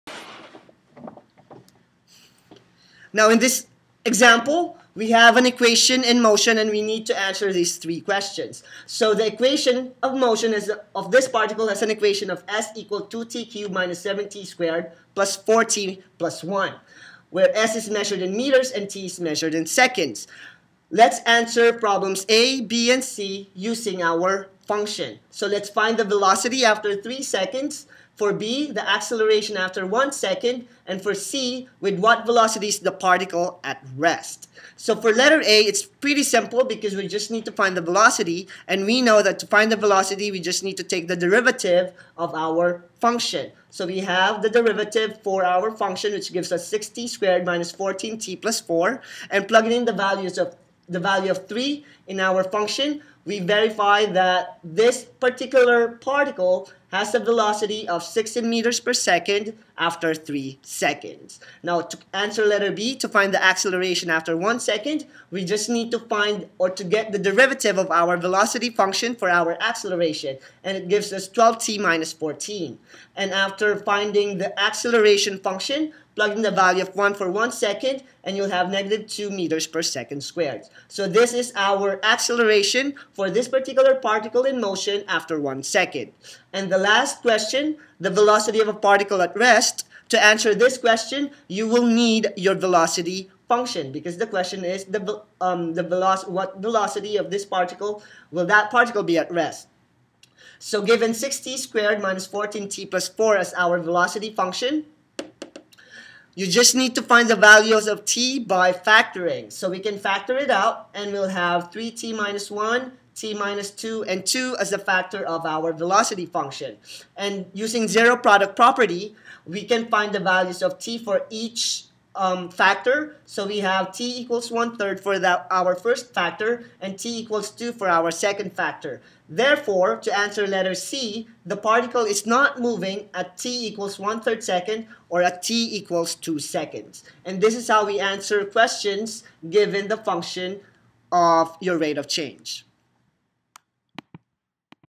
Calculus: Application of Derivatives Velocity and Acceleration
This lesson on calculus shows you how to solve problems involving acceleration and velocity of a moving particle or object on a straight line.  the first derivative and the 2nd derivative of the functions will represent these type of movements.